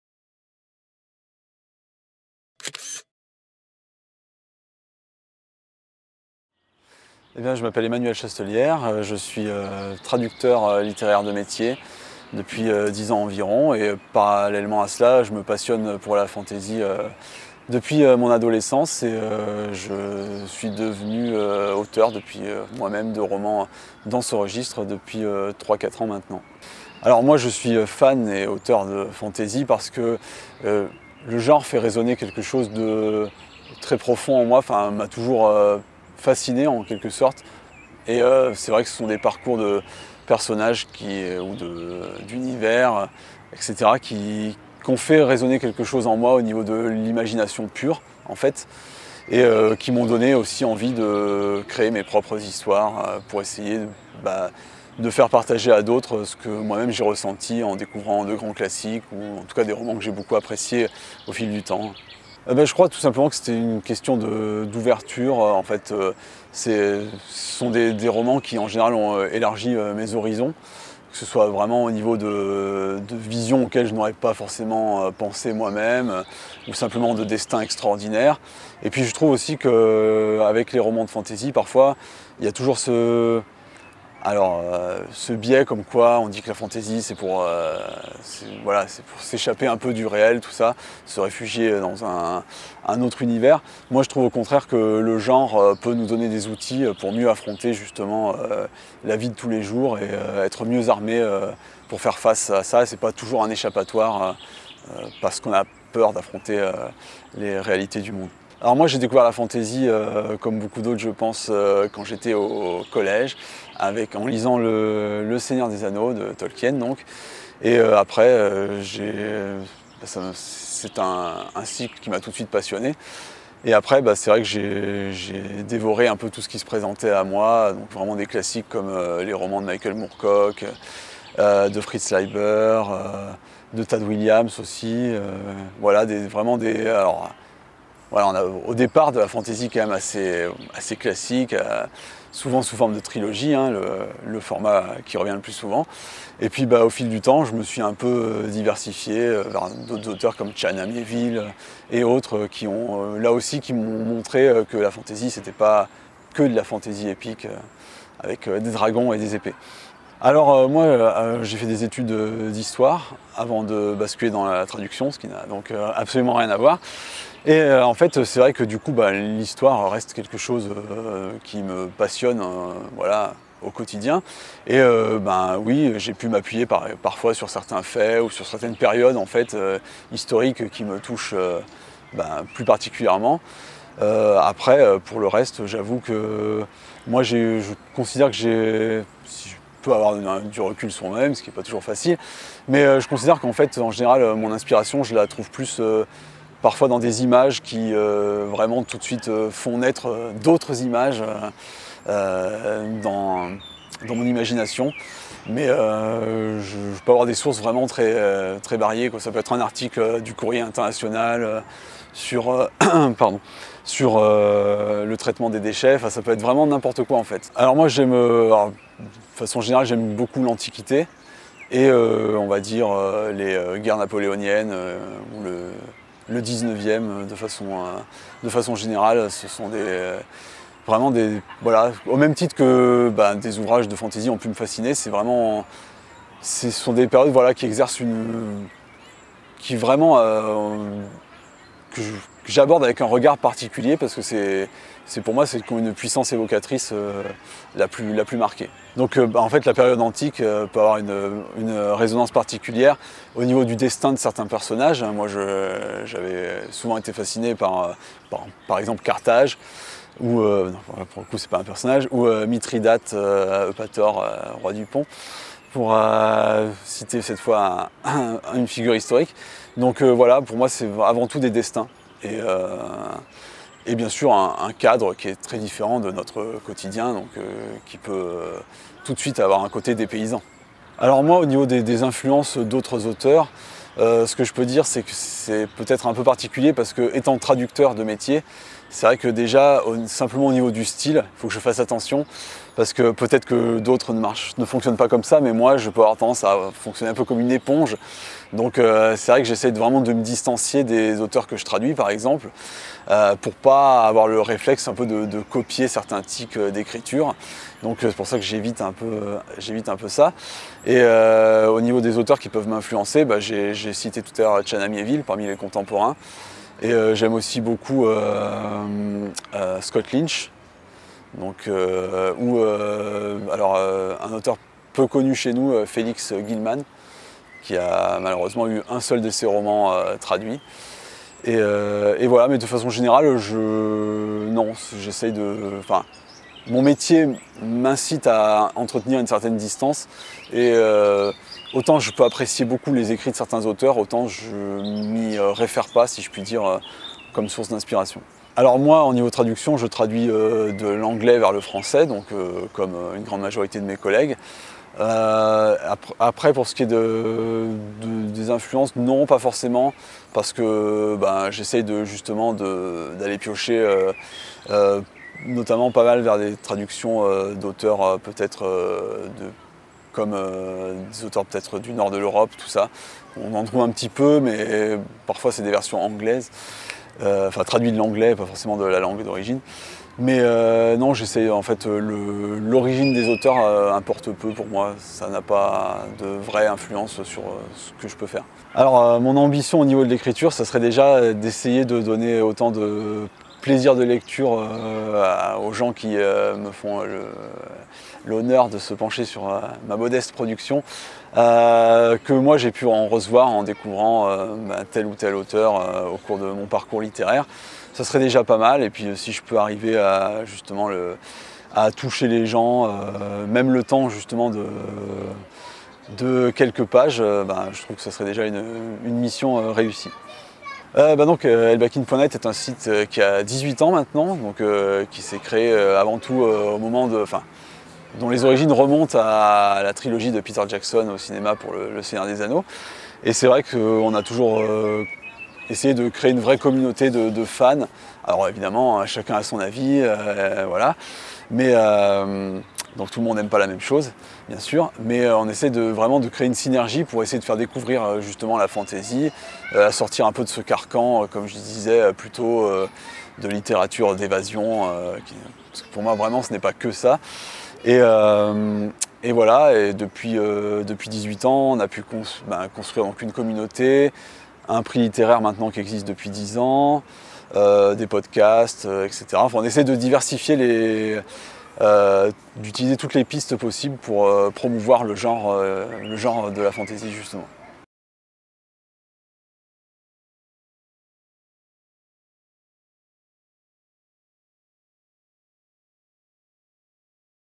La Fantasy en France #8 : Emmanuel Chastelliere
Emmanuel Chastelliere, auteur de romans de l’imaginaire et traducteur nous parle de son travail, ses influences et sources d'inspirations.